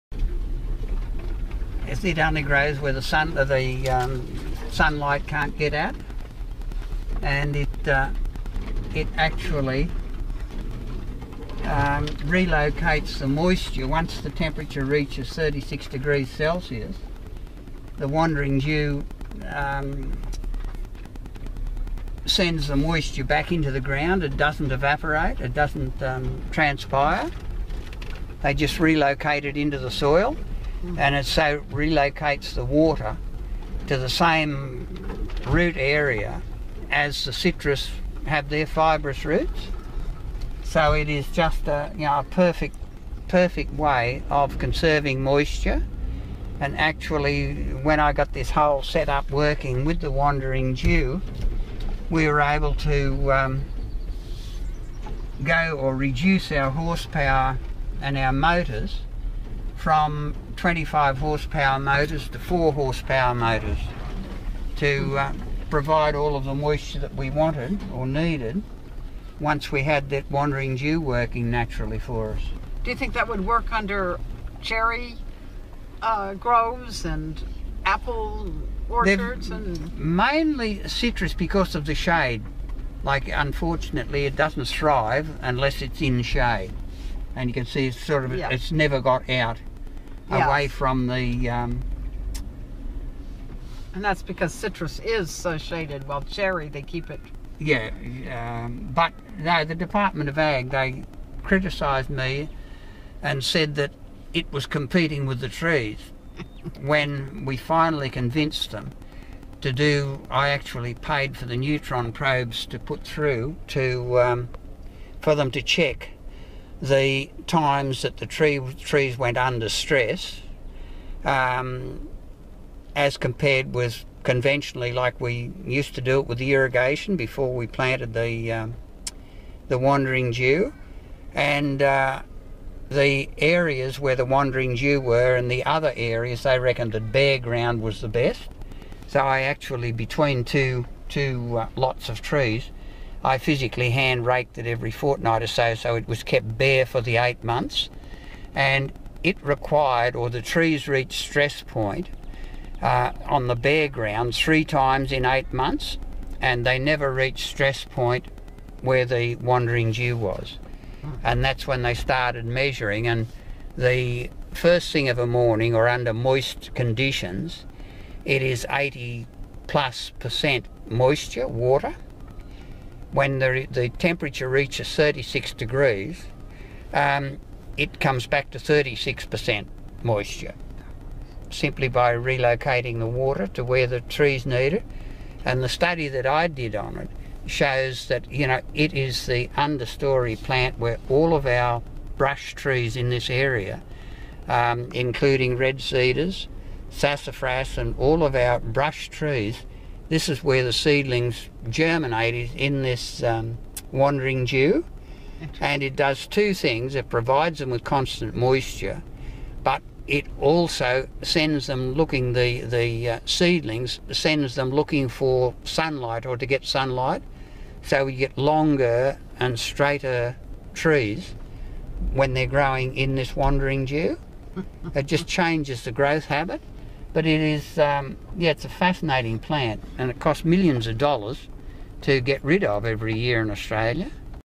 citrus irrigation
Biodynamic expert John Priestley explains how he uses wandering jew plant to reduce his citrus grove irrigation by half.